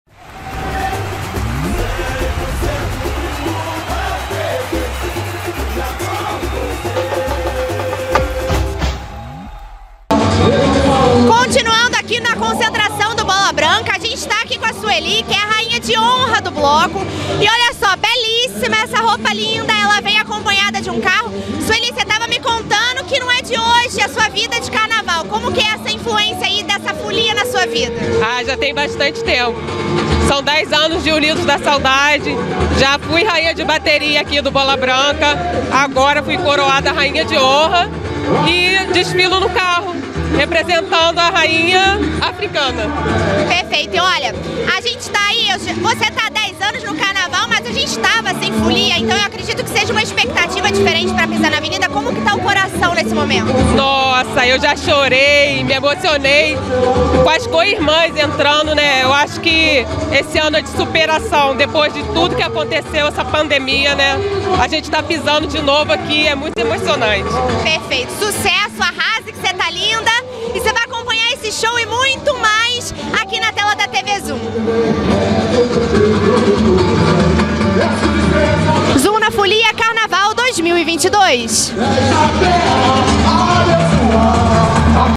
Zoom na Folia 2022 - Bola Branca - Rainha de Honra
Conheça a rainha de honra do Bola - Branca! Assista AO VIVO. Hoje, dia 13 de maio, o desfile das escolas de samba da Série A. E amanhã, dia 14 de maio, os desfiles do Grupo Especial. Zoom na Folia 2022!